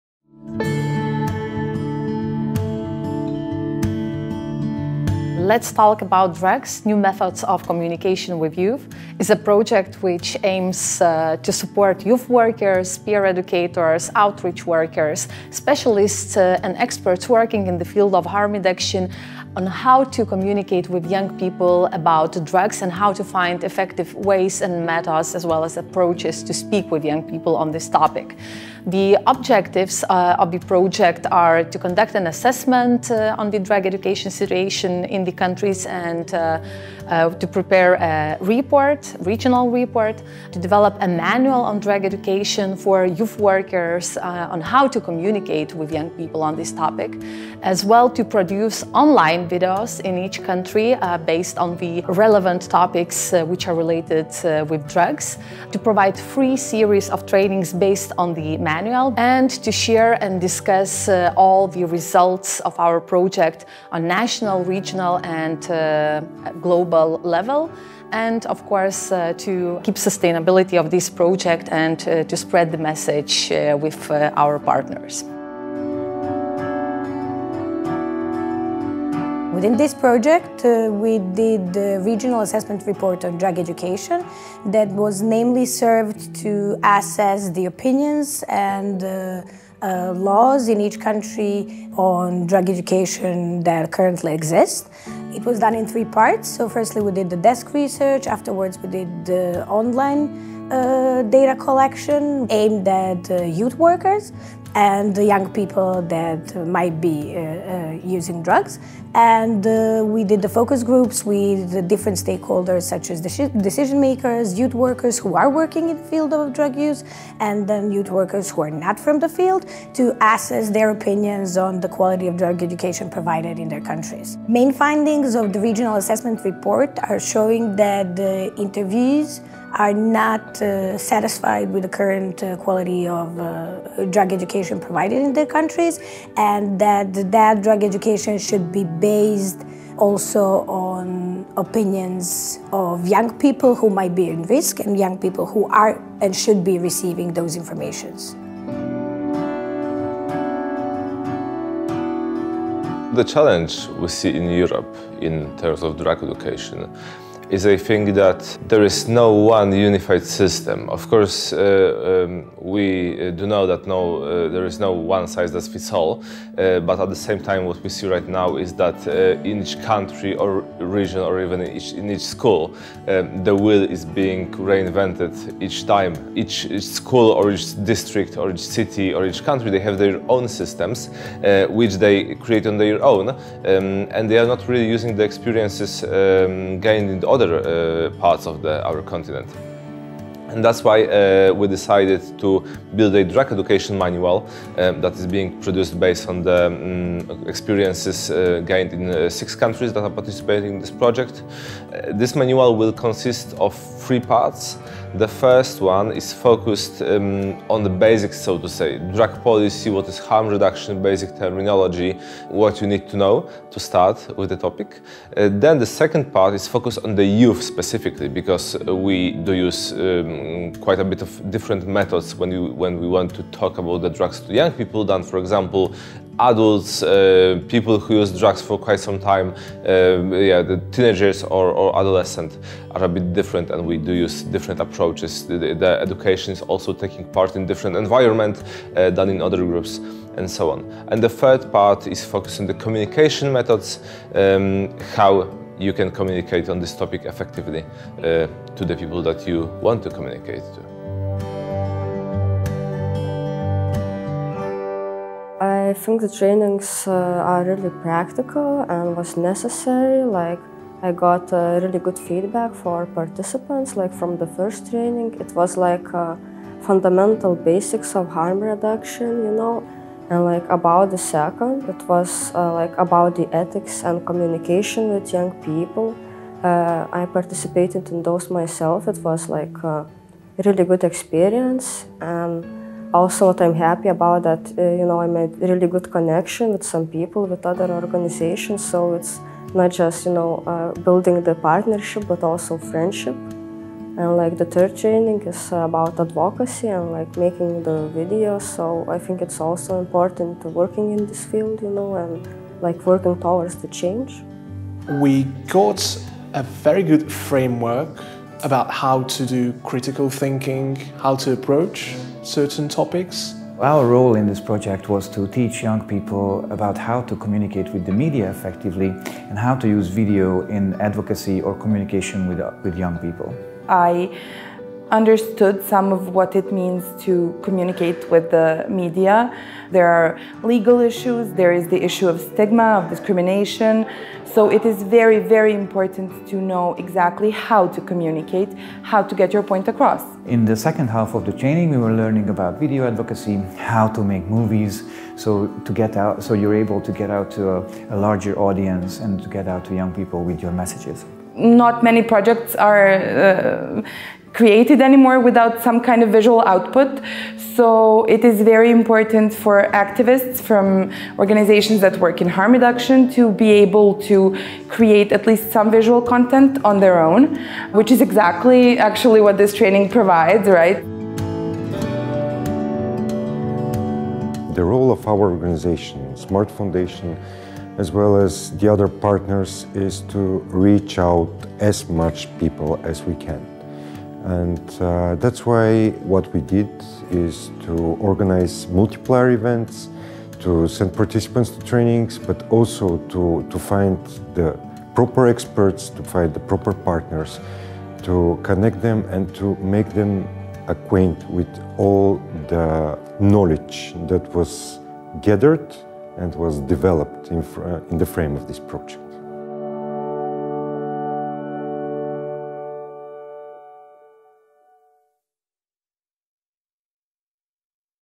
LEt’s Talk about drugs – new MEthods of communication with youth (LET ME)
This short video summarises the activities of the "LEt’s Talk about drugs – new MEthods of communication with youth (LET ME)" Erasmus+ project. The aim of the project is to support youth workers, educators (peer educators), street workers, harm reduction specialists working directly with youth who can potentially start using drugs or already use them with the innovative approaches and methods on drug education. The main target group of the project will be youth workers, youth educators, peer educators and outreach workers who will be able to influence the young people who use drugs, directly within the outputs of the project. more information is available